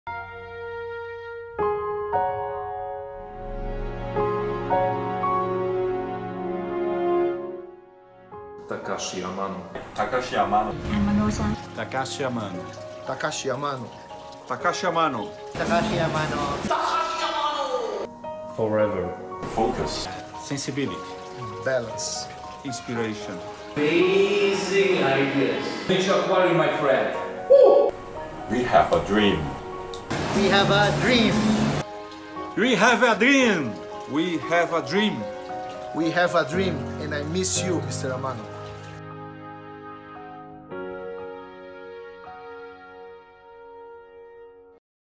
Takashi Amano - Celebrating the Master and his Teachings
Celebrating the master and his teachings, here is a sneak peak into the world of his fans. Like them there are thousands around the world who have been inspired by him and his work.
How would you describe Takashi Amano in one word ?

Detailed Fan videos coming soon.